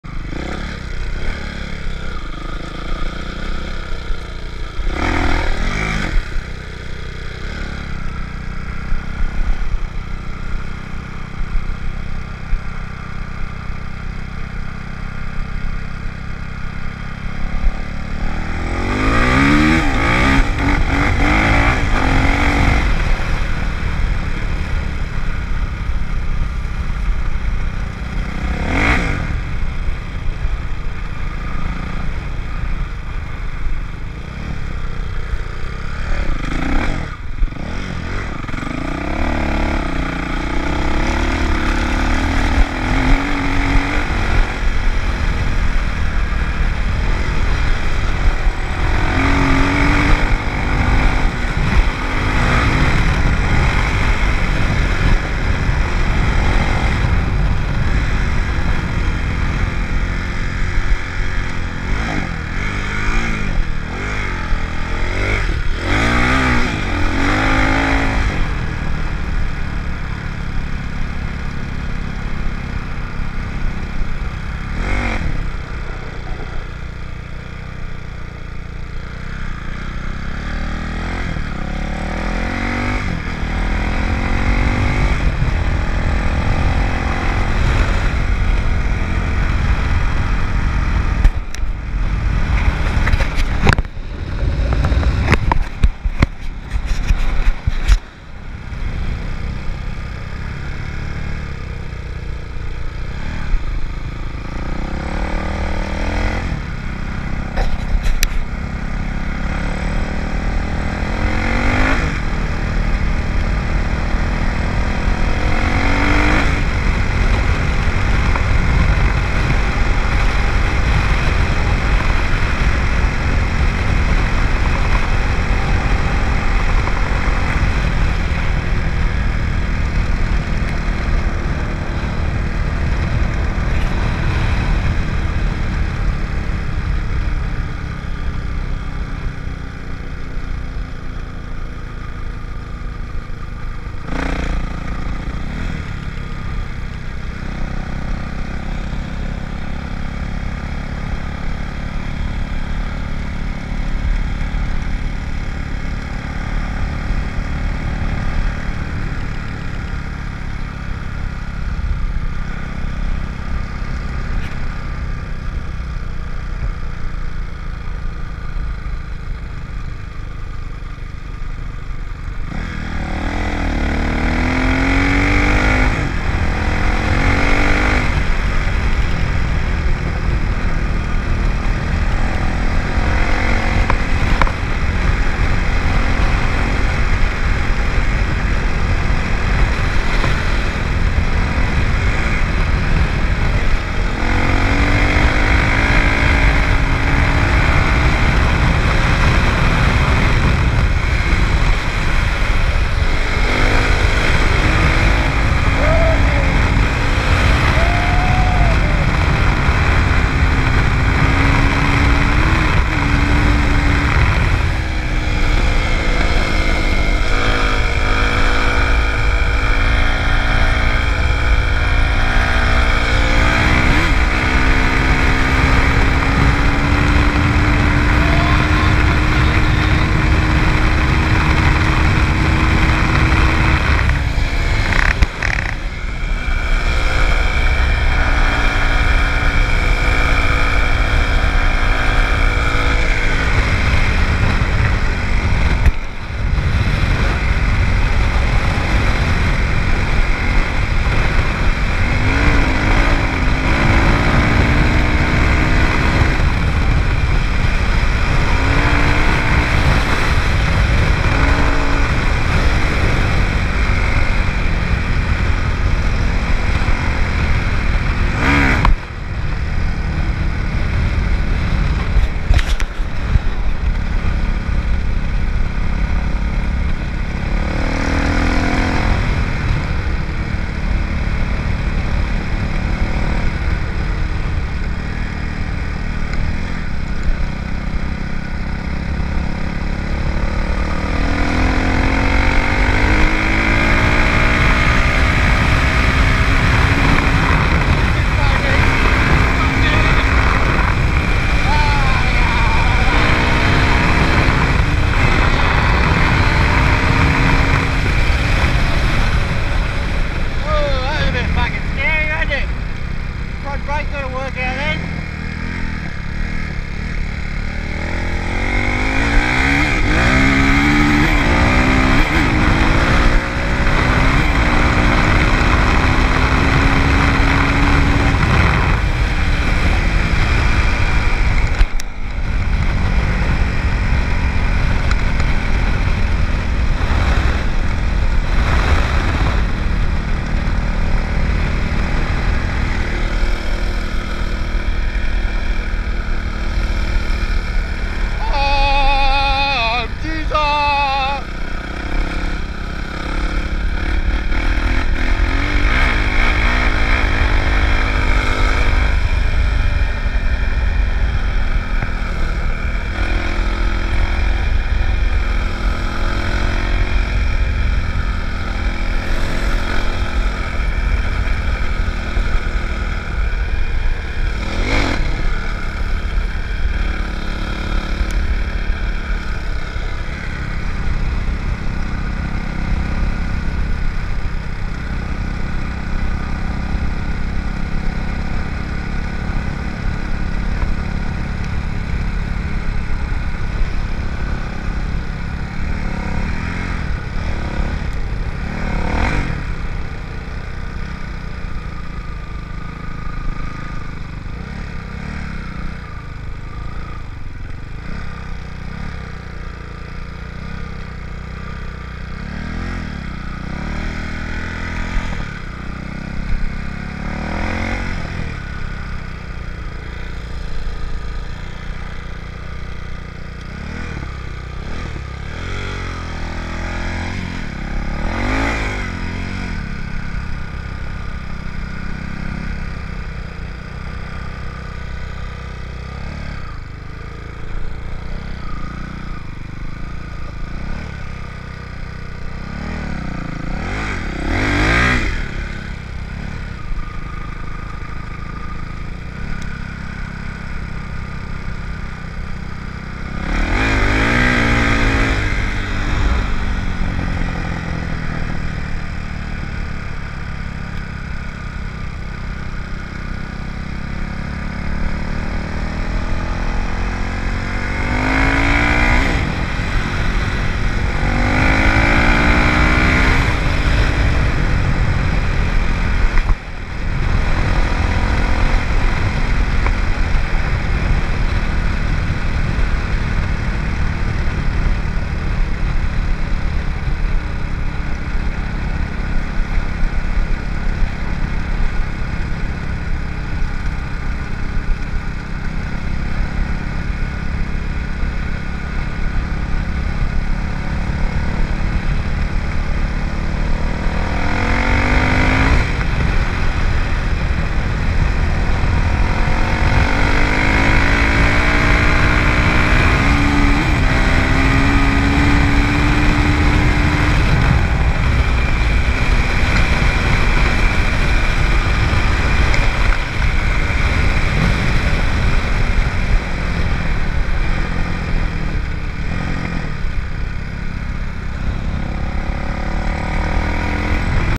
another squirt on the KLX 450r
Kawasaki KLX450r , still recovering from a dislocated shoulder and fractured collarbone so I only opened her up a few times.